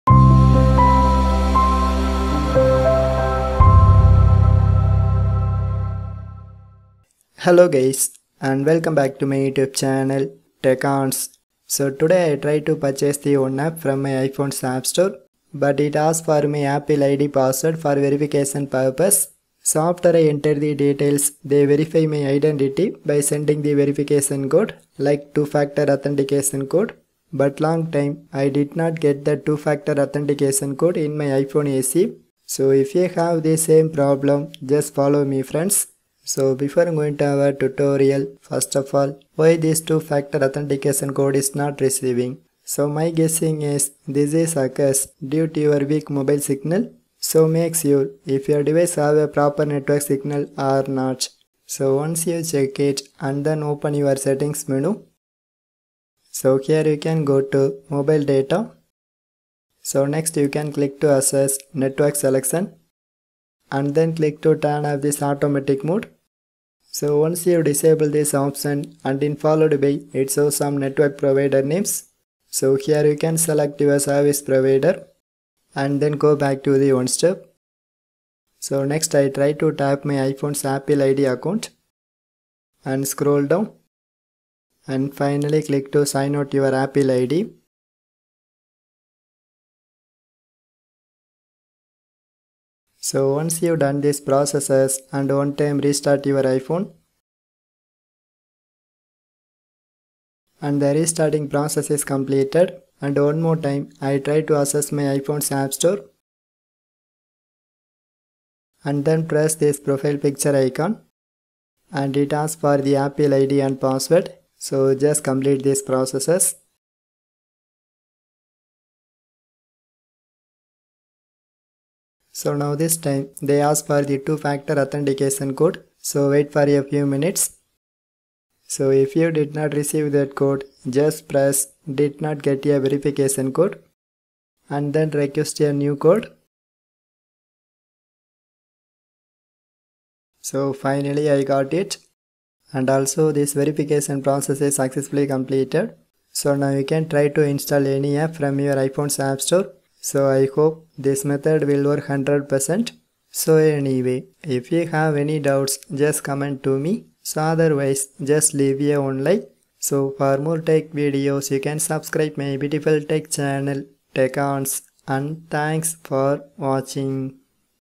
Fix iphone two factor authentication code not received/not sending | problem solved
This tutorial is about how to fix iphone two factor authentication code not sending or verification code not received problem.

Here i show the steps by using my iphone se and which is have the operating system of 2023 released ios 15.4.1.

But if your phone is missing or lost,you can change your two factor phone number using apple website.

At same time if you enter the wrong number or if you request too many codes,this method not working on your ipad also.

This video has english subtitle at same time you can translate to tamil,kaise kare hindi,bangla telugu,kannada,cara mematikan,cara mengatasi and malayalam languages.

Today i get the following one error notification while i try to sign in my iphone's apple id :

Two-Factor Authentication

A message with a verification code has been sent to. Please enter the code to continue.

Did not get a verification code?

Send new code

Get a phone call

Cannot use this number

Cancel

How to solve iphone two factor authentication not getting code :

1.One of my subscriber ask to me,why i can't get this verification sms and what is the meaning of this issue?.

2.Some time this is occurs if your mobile not have a proper network signal.

3.So first of all check your mobile signal.

4.Once you check it and one time sign out your existing apple account.

5.And don't forget to restart your iphone.

6.So after the restarting is completed,now you can try to sign in your account in iPhone app store.

7.So i hope if you follow this simple steps,finally this code not received problem is solved in permanently.

8.But there is no possible to bypass this two step verification.

9.By using this recovery method you can easily turn off this 2 factor authentication setting.